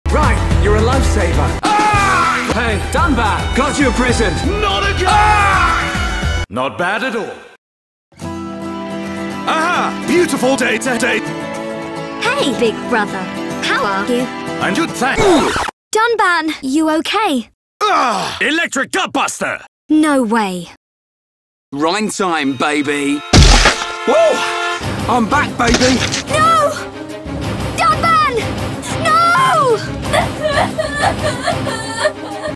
Dunban Get's A Tummy Ache
Appreciate you all watching!
How's this for my first video of 2022? 😂

This is a continuation of "Shulk Get's Dunban A Gift" video so watch that one first

After the events of the previous video, Dunban goes for a walk and run's into Fiora where he suddenly has a stomach ache. The lifesaver's effects are wearing off and let's just say that things get messy.

If your enjoying this storyline and want to see more of this story such as what happens next or how Shulk turned Zeke into a lifesaver, then leave a comment below letting me know 😊